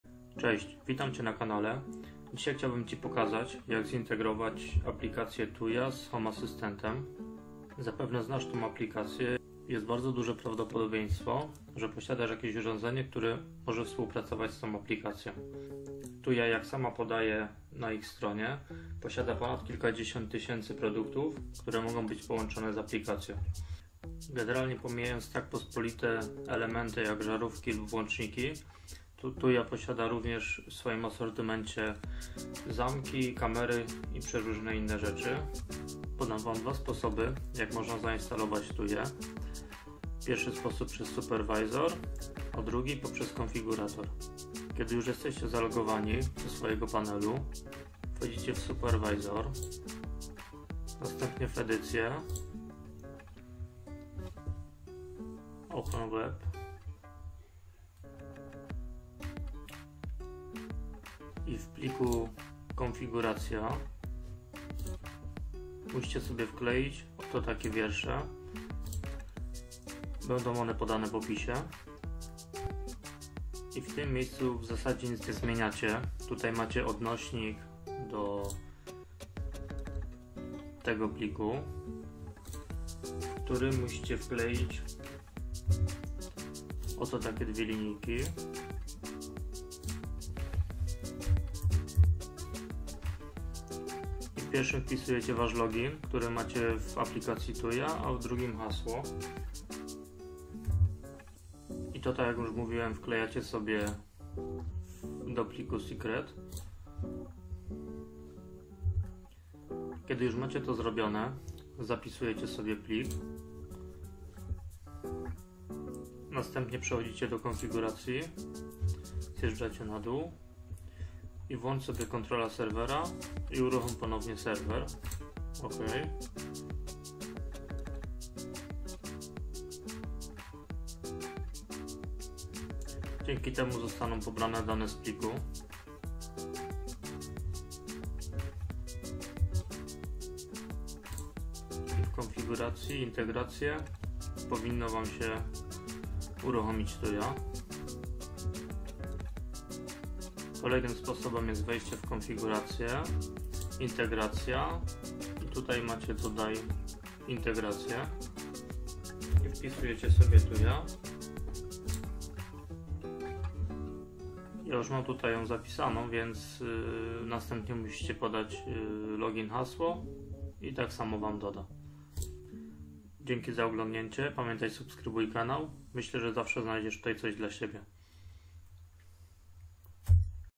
Jak Uruchomić Tuya w Home Assistant - Integracja Konfiguracja
Witaj w tym odcinku zobaczysz jak uruchomić TUYA w środowisku Home Assistant dzięki czemu będziesz mógł za pomocą wifi lub zigbee połączyć swoje urządzenia ze swoim Home Assistant. Z aplikacją TUYA można skonfigurować kilka tysięcy urządzeń co stwarza to środowisko stosunkowo konkurencyjne a ceny urządzeń są rozsądne.

Pomogło? Postaw kawę :)